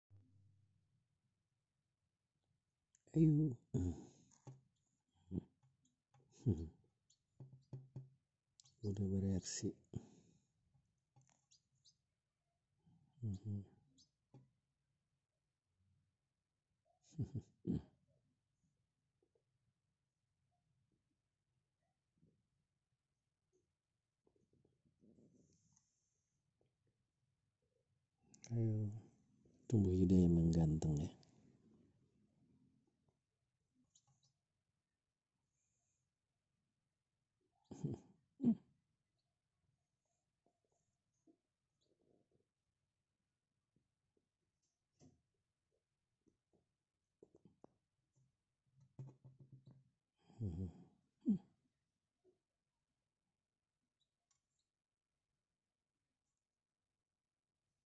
[bag. 6] selamat datang di duniamu yg baru, Bule..
yaapps..
pada akhirnya, si Bule pun bisa melangsungkan hidupnya di dunianya yg baru.